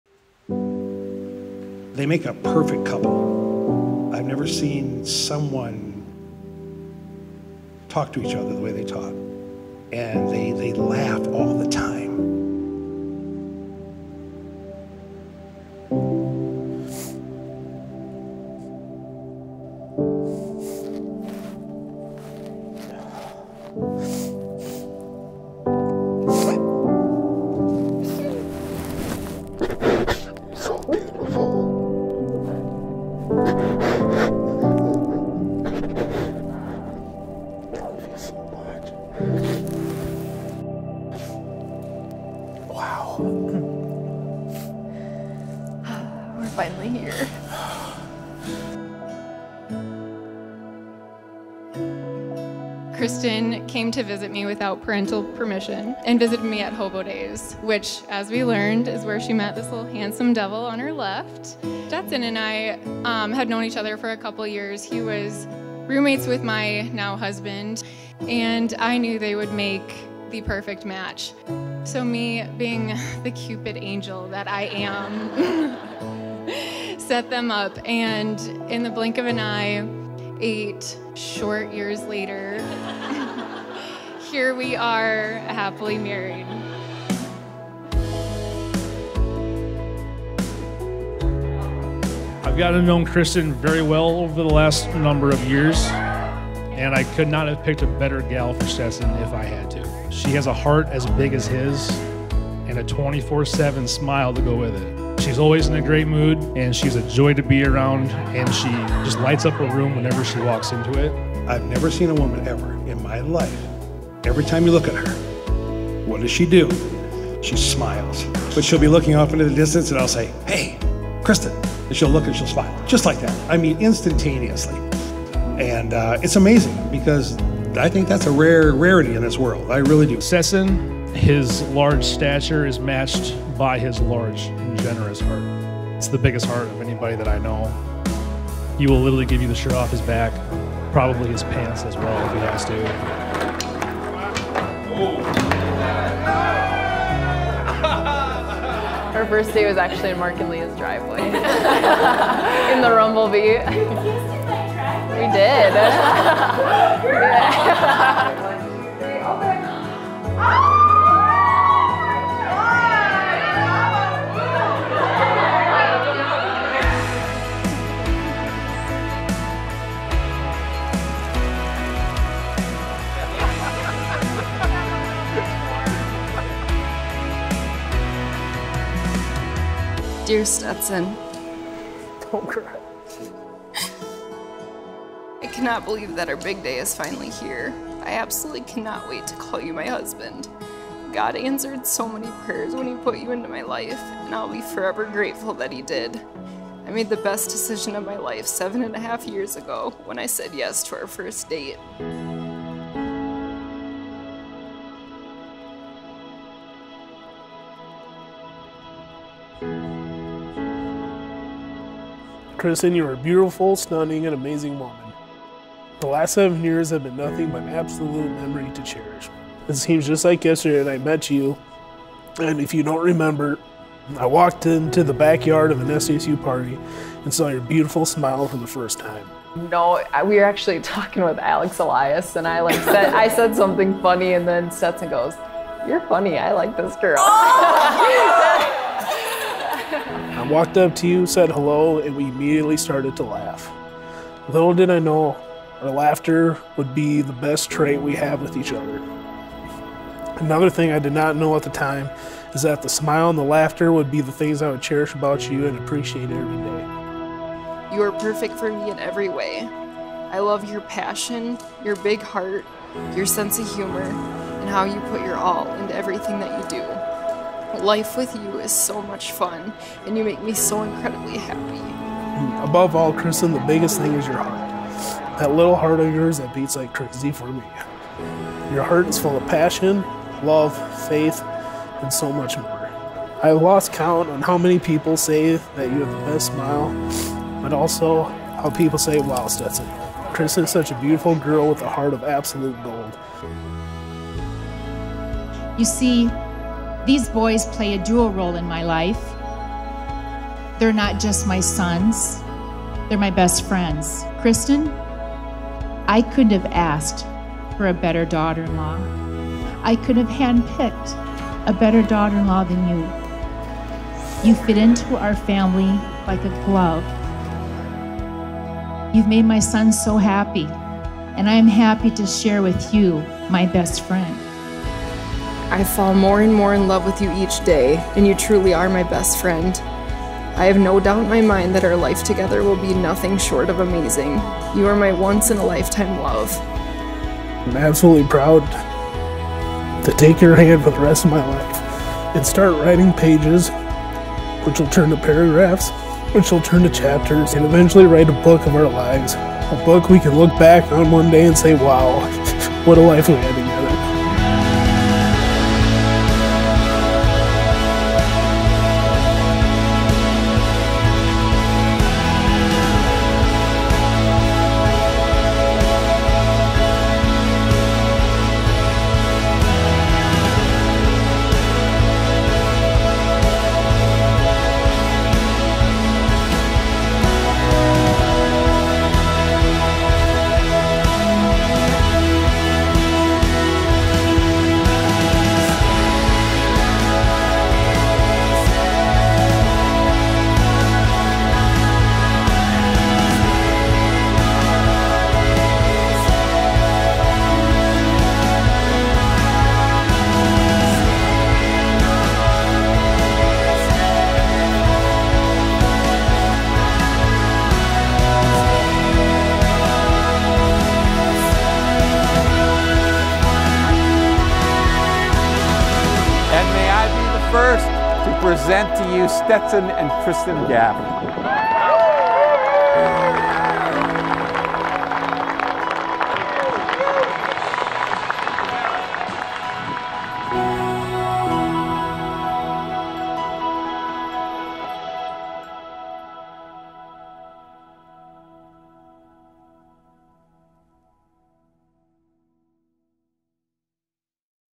Groom Cries When He Sees His Bride | The Atrium Wedding Venue
Kristen and Stetson got married in Sioux Falls, SD at The Atrium by Blue Haven Barns. Kristen and Stetson met in college and hit it off right away. When Stetson met Kristen he was taken back by her laughter and her smile, little did he know they would be his favorite things about her.

We would love to be your wedding videographer!